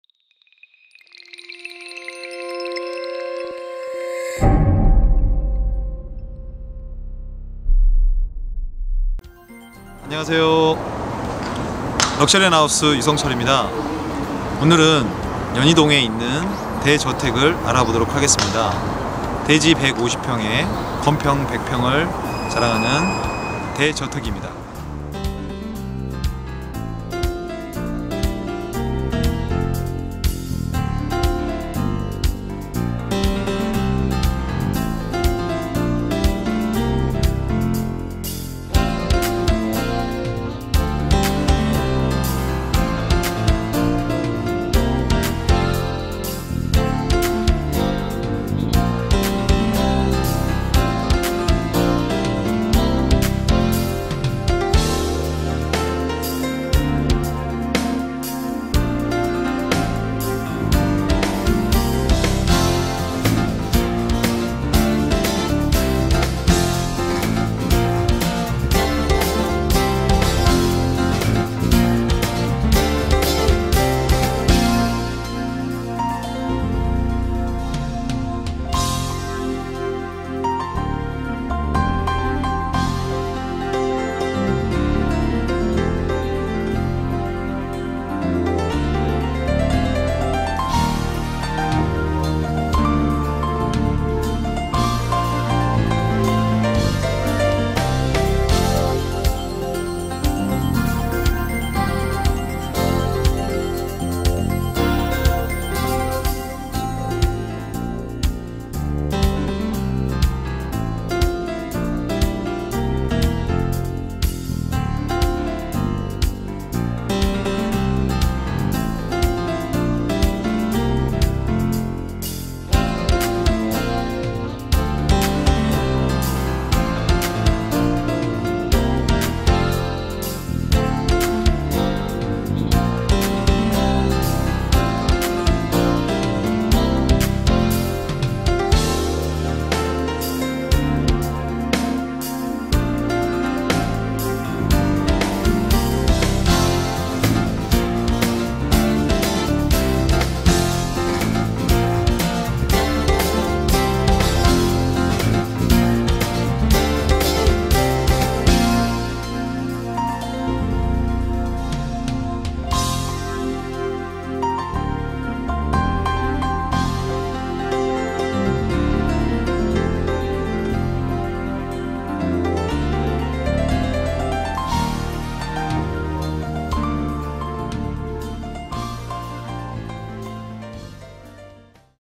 연희동 복층형 단독주택 대지 149평 연면적 117평 주차 4대 방 6 화 4 금액 사이트 참조
▶ 소개해드리는 연희동 단독주택은 서울 외국인학교와 인접해 있는 곳으로 2010년에 지어진 복층형 단독주택
▶ 정림건축에서 설계하여 지은 집으로 공간 제한 없이 각 공간 마다 채광과 전망이 탁트인 설계가 특징이며 다락방, 차고, 창고 등 다용도 공간도 넉넉하게 마련되어 있습니다
▶ 연희동에서도 안자락에 위치하고 있어 공기가 좋으며 언덕에 자리하고 있어 내부 어디에서든 탁 트인 전망을 감상 하실 수 있습니다
▶ 거실, 주방, 룸 까지 모두 공간이 넓게 마련되어 있으며 방수도 많아 대가족이 함께 생활 하기에도 좋은 연희동 단독주택
▶ 유명 건축사무소인 이화대학을 설계한 정림건축에서 지은 집으로 룸과 욕실마다의 채광과 전망을 고려한 설계가 특징이며, 다락방, 경비실, 주차 4대 공간, 창고, 많은 룸갯수 등 사용할 수 있는 면적이 넓은 특징이 있는 연희동 단독주택
▶ 또한 서울외국인학교가 바로 접하고 있고, 한성화교학교(중국인학교)가 주변에 있어 매입 후 외국인렌트 수익률을 충분히 기대할 수 있는 물건으로 매매 하시는 방법도 적극 권유해드립니다

브랜드명 : 럭셔리앤하우스
대표: 김남훈, 등록번호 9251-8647
중개법인 위치 : 서울시 강남구 언주로 174길 23(신사동, 동부저축은행 3층)